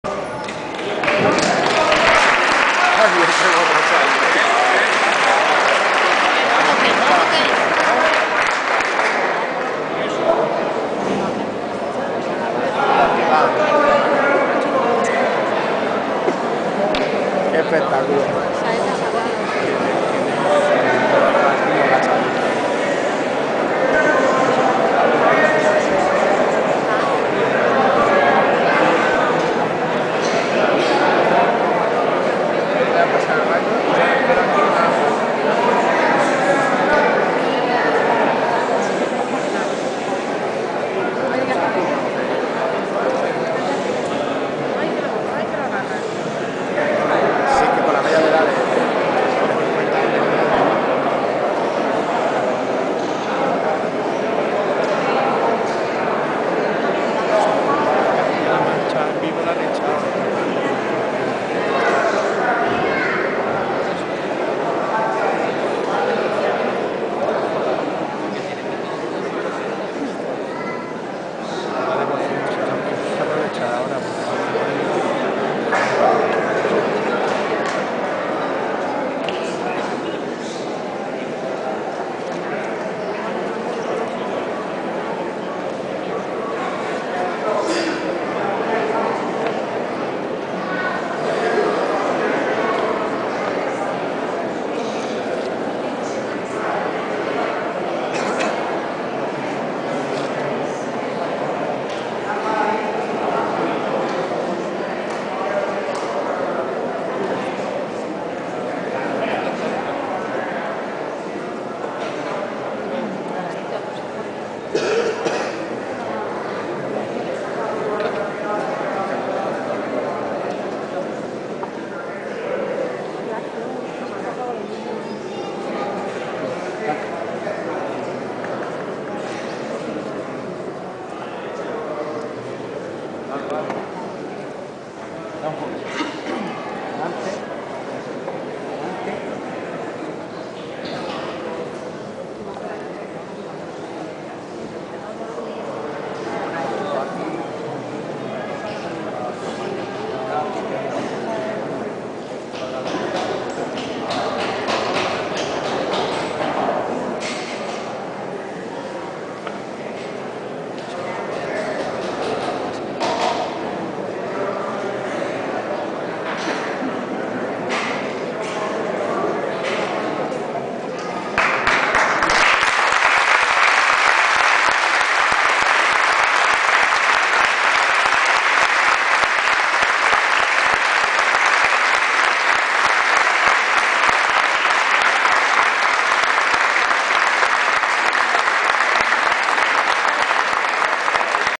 PROCESIÓN DEL SANTÍSIMO CRISTO DE LA SANGRE,TORRIJOS 19-5-2009 VII
Subida al altar del Santísimo Cristo de la Sangre, de Torrijos (Toledo) al finalizar su procesión el 19 de mayo de 2009